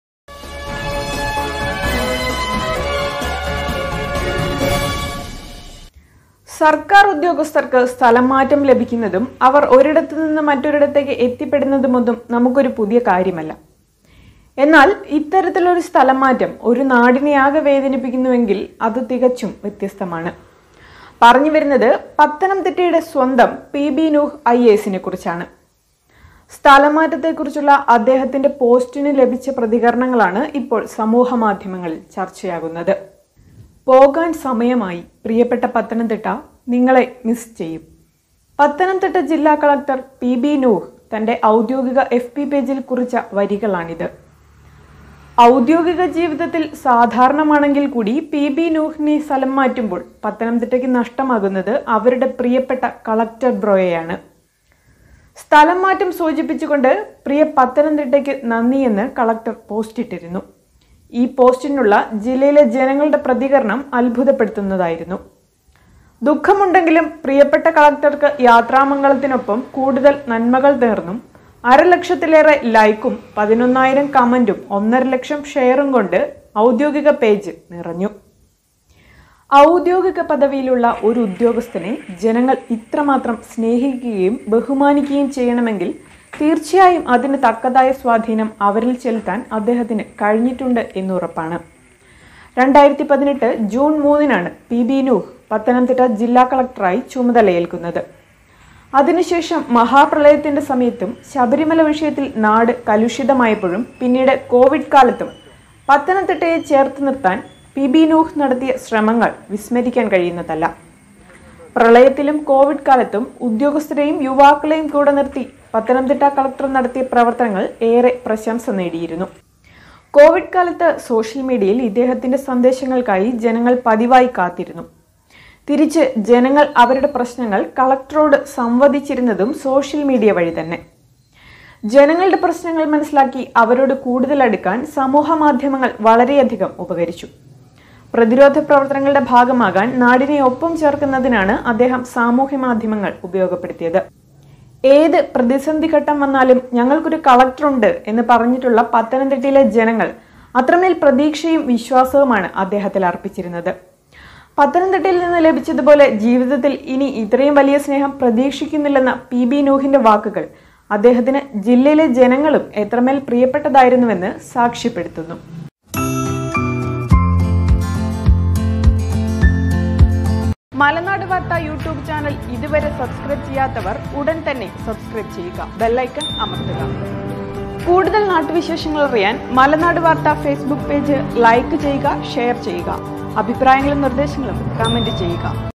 പോകാൻ സമയമായി, പ്രിയപ്പെട്ട പത്തനംതിട്ട, നിങ്ങളെ മിസ് ചെയ്യും'
സർ്ക്കാർ ഉദ്യോഗസ്ഥർക്ക് സ്ഥലം മാറ്റം ലഭിക്കുന്നതും അവർ ഒരിടത്തുനിന്ന് മറ്റൊരിടത്തേക്ക് എത്തിപ്പെടുന്നതുമൊന്നും നമുക്ക് ഒരു പുതിയ കാര്യമല്ല. എന്നാൽ ഇത്തരത്തിലൊരു സ്ഥലംമാറ്റം ഒരു നാടിനെയാകെ വേദനിപ്പിക്കുന്നുവെങ്കിൽ അത് തികച്ചും വ്യത്യസ്തമാണ്. പറഞ്ഞുവരുന്നത്, പത്തനംതിട്ടയുടെ സ്വന്തം പി ബി നൂഹ് ഐഎഎസിനെക്കുറിച്ചാണ്. സ്ഥലംമാറ്റത്തക്കുറിച്ചുളള അദ്ദേഹത്തിന്റെ പോസ്റ്റിനു ലഭിച്ച പ്രതികരണങ്ങളാണ് ഇപ്പോൾ സമൂഹമാധ്യമങ്ങളിൽ ചർച്ചയാകുന്നത്.